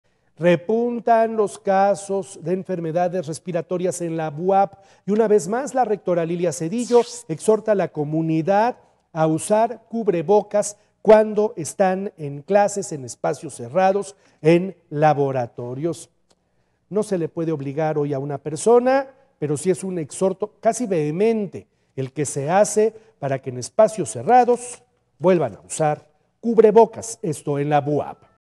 Repuntan casos de enfermedades respiratorias BUAP || Noticias con Juan Carlos Valerio
Entérate 😷 || Tras un repunte de casos de Covid-19, la Rectora de la BUAP, Lilia Cedillo pide a los estudiantes utilizar cubrebocas en espacios cerrados.